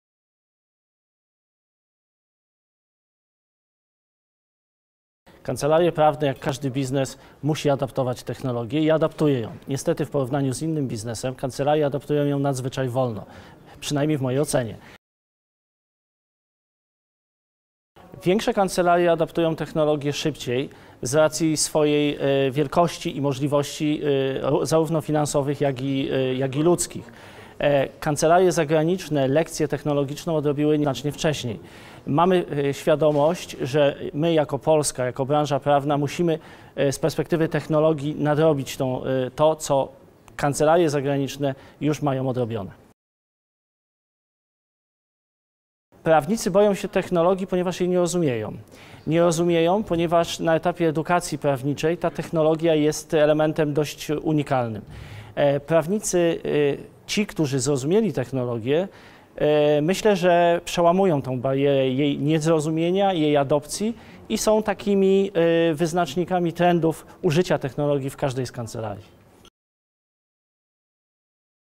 Postęp technologiczny w kancelariach w Polsce – Rising Stars 2020
O wpływie technologii na kancelarie w Polsce, postępie technologicznym i o tym czego boją się prawnicy opowiada Marek Laskowski szef IT w Kancelarii Domański Zakrzewski Palinka.